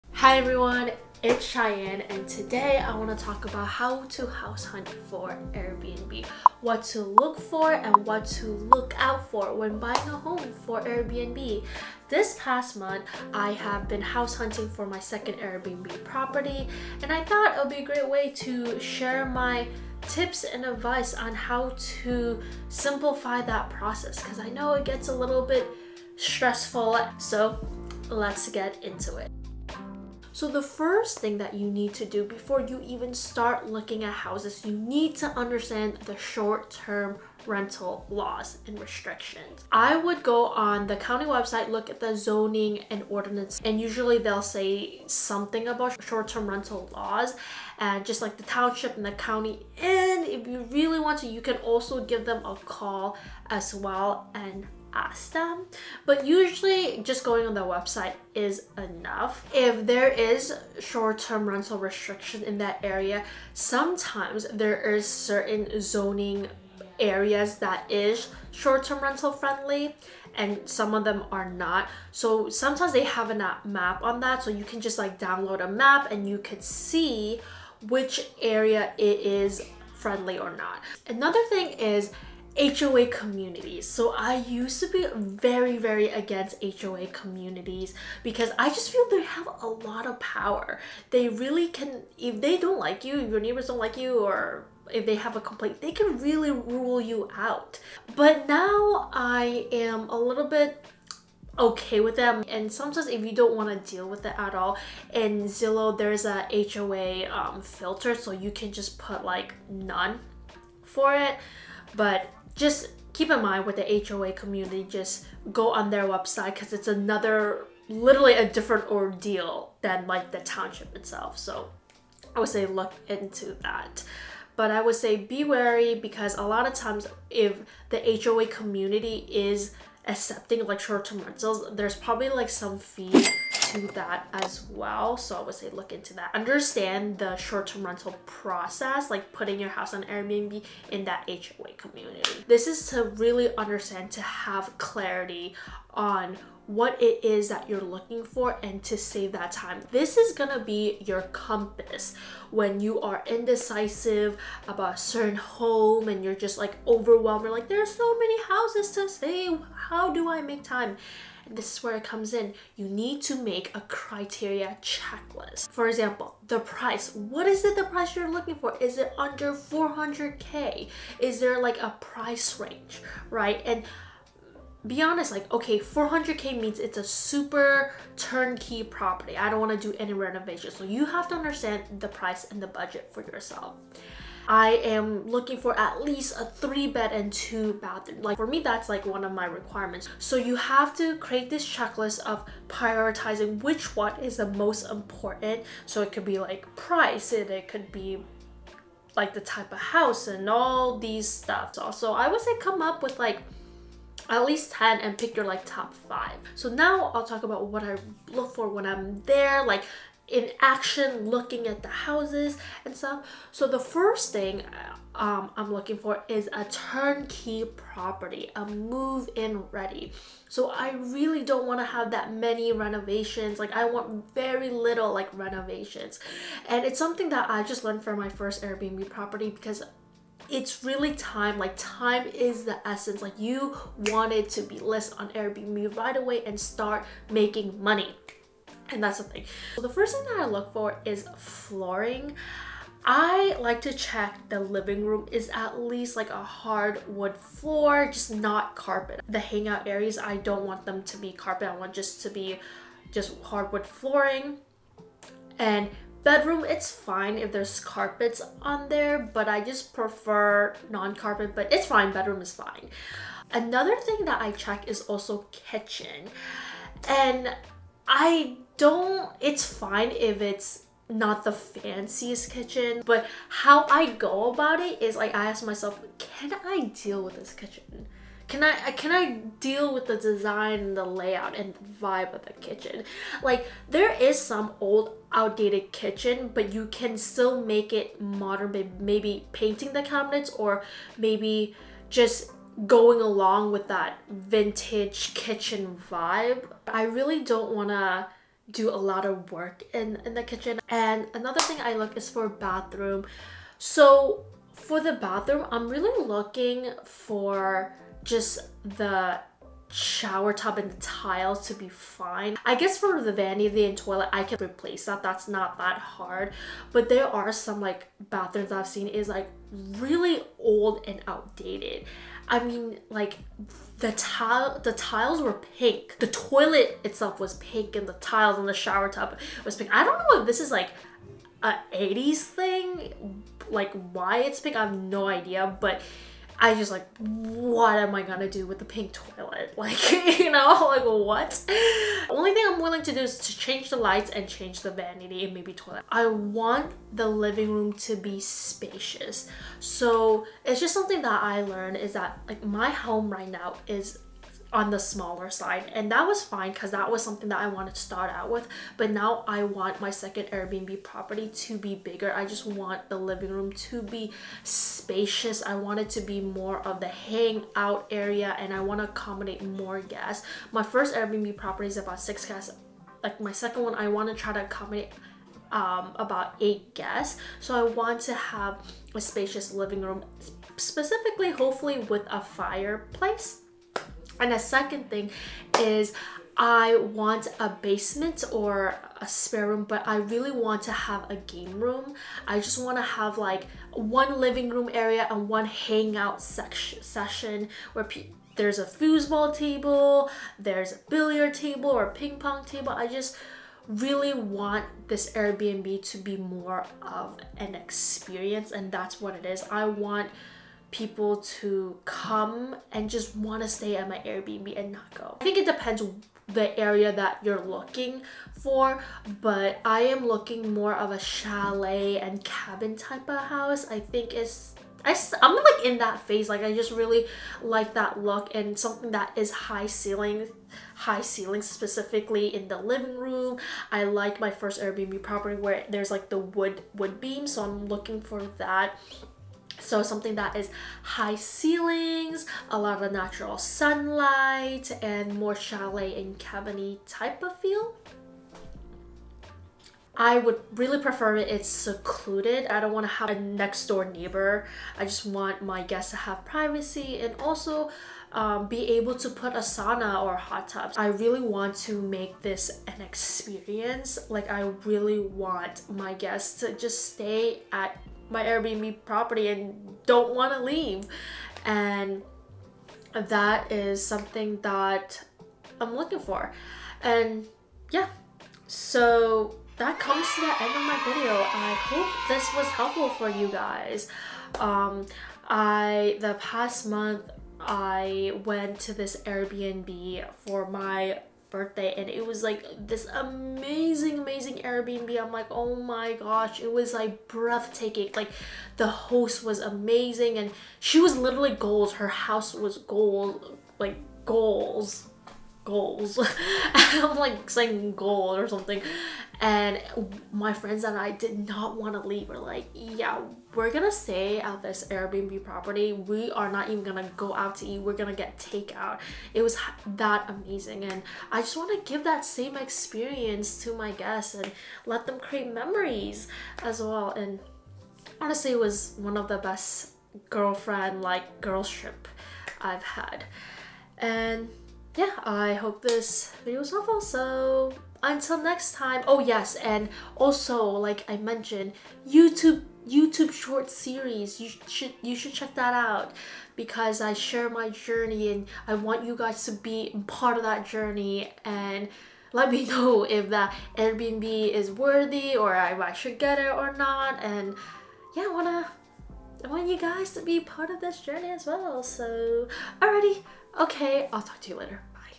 What to look for when buying a home for Airbnb
Hello it's Shyenne and I'm back with a video on what to look for and look out for when buying a property for Airbnb. I share my tips and advice on how to house hunt Airbnb and simplify the searching process.

Video Chapters
00:00 Intro
00:30 Short-Term Rental Restrictions
02:22 Criteria Checklist
03:26 Turnkey property
05:45 Spacious Common Room
06:55 Exterior